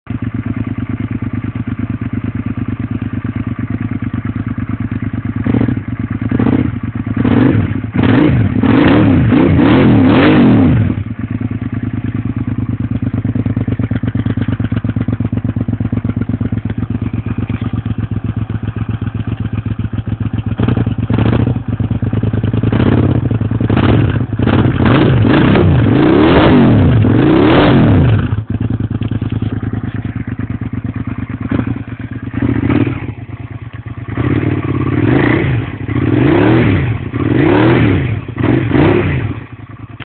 SV650 Yoshimura TRS
Here's a little video of my '01 SV650S... full Yosh TRS exhaust, desnorkeled air filter and rejetted.